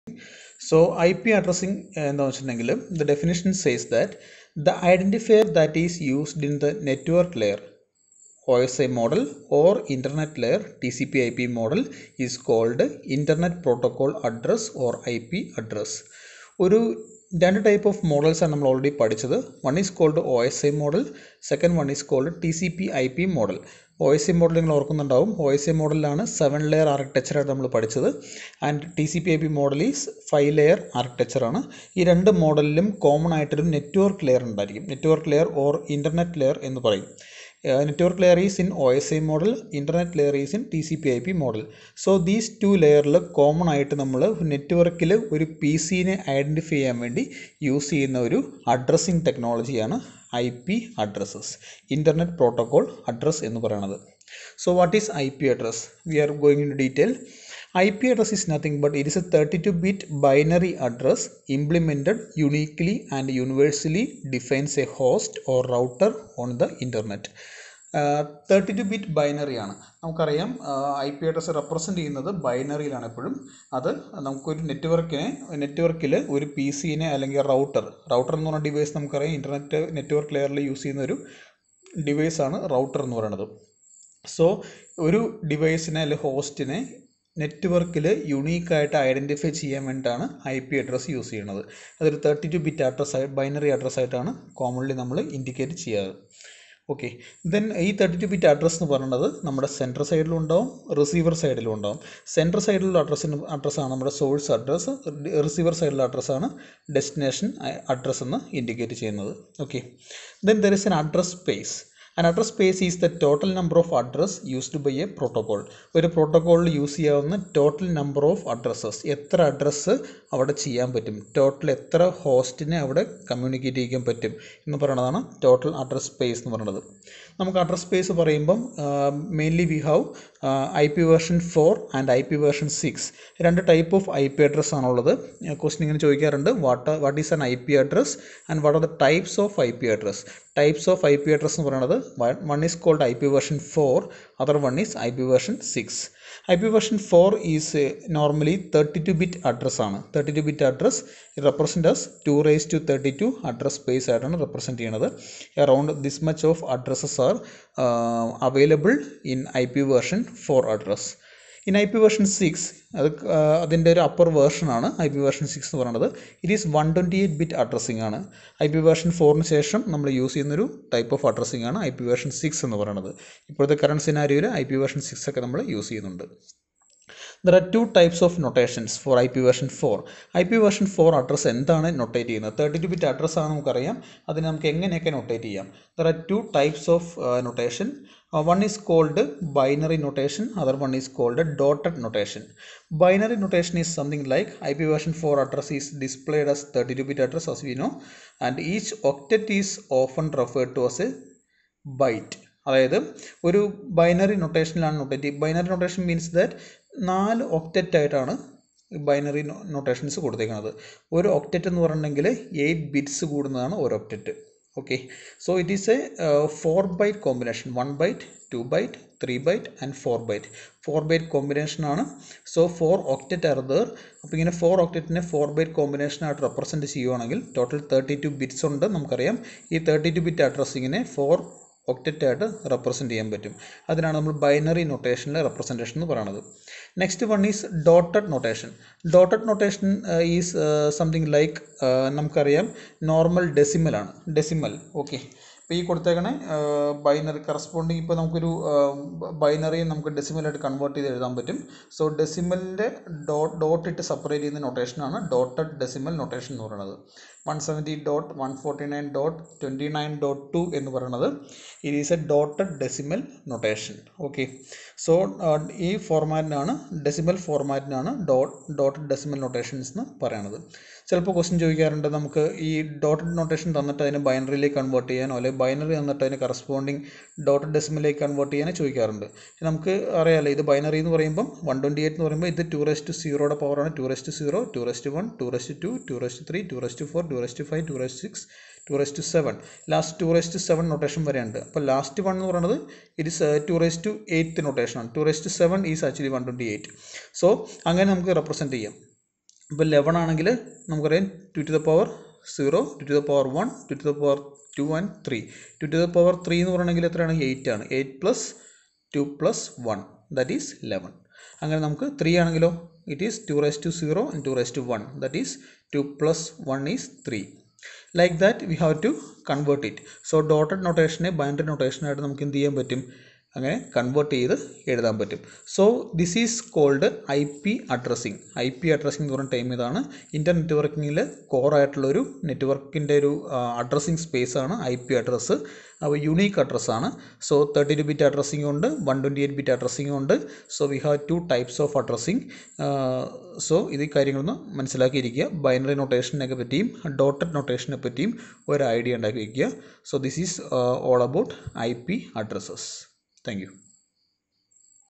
CS 306: Computer Networks- Module 4: IP Addressing
This video deals with IP Addressing and its representation.